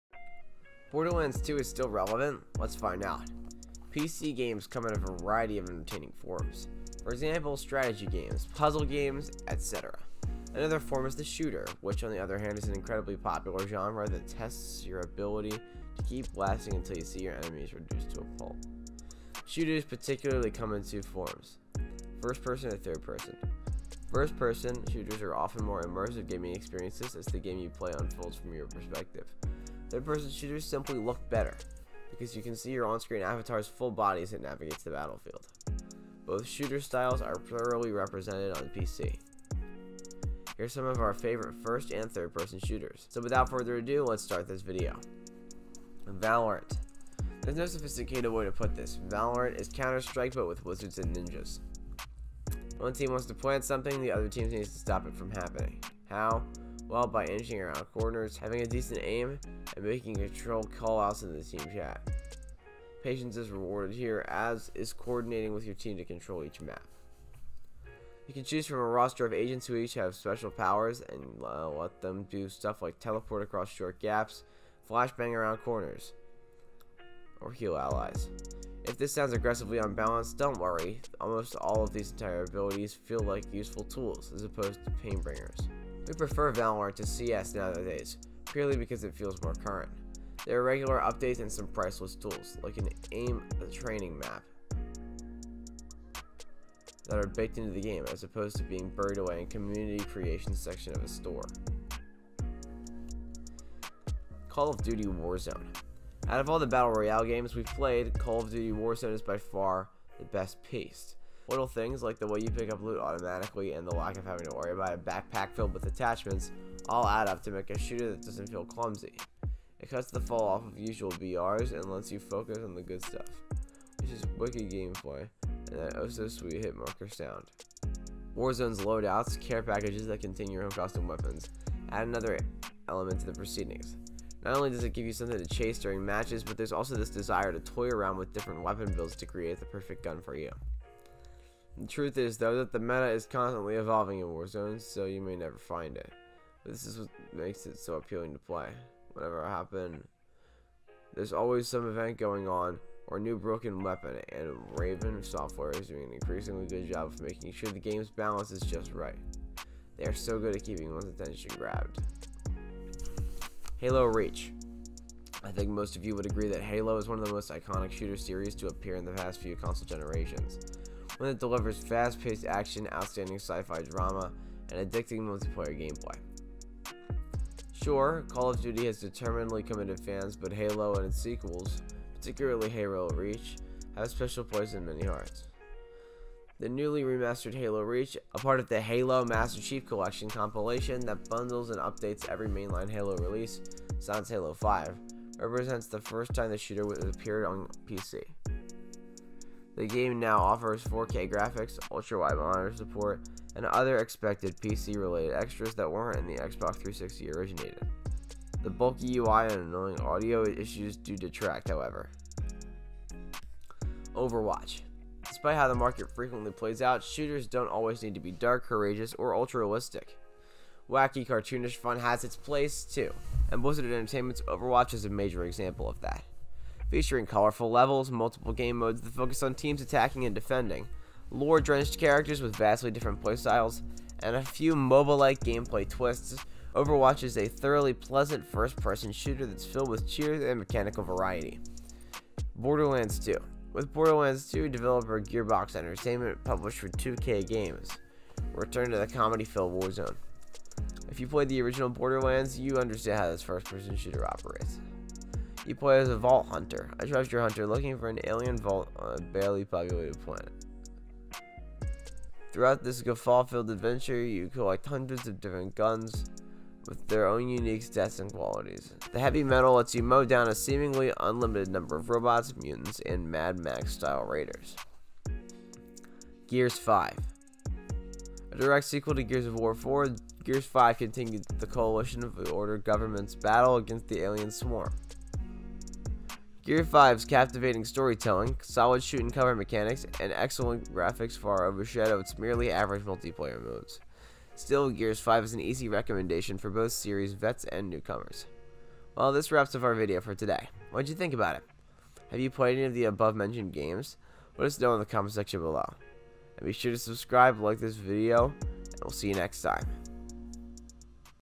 The Best PC Shooter Games in 2022!
In this video, we break down The Best PC Shooter Games in 2022!